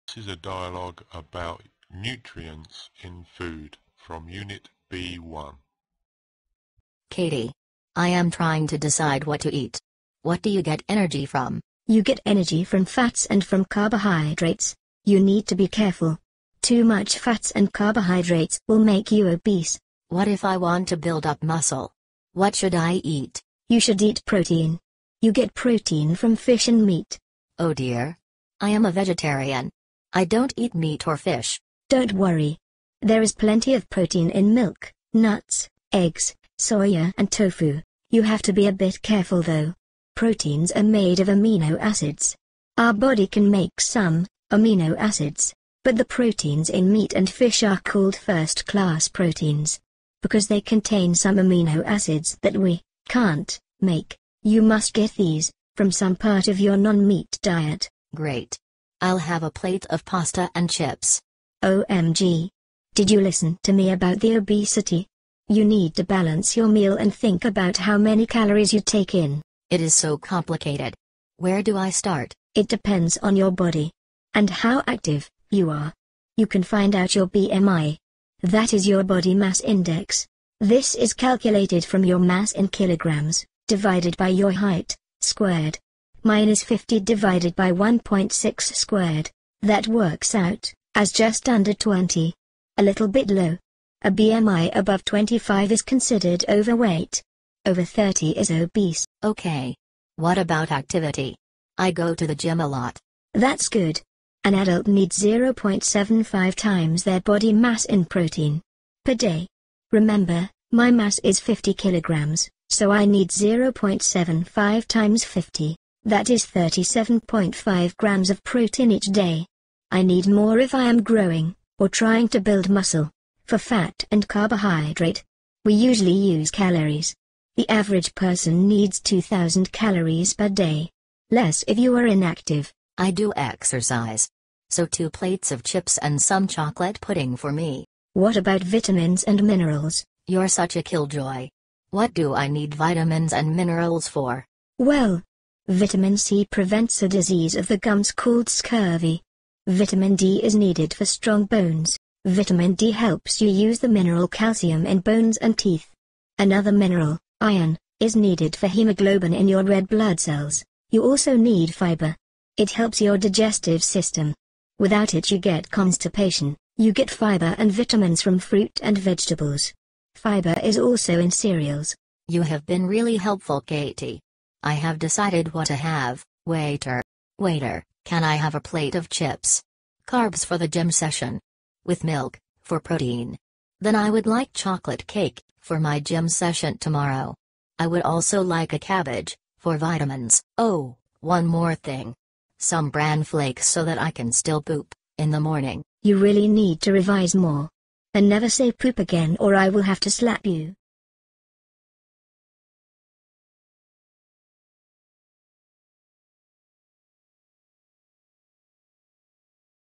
Nutrition B1 Revision
A GCSE science revision video about nutrients in food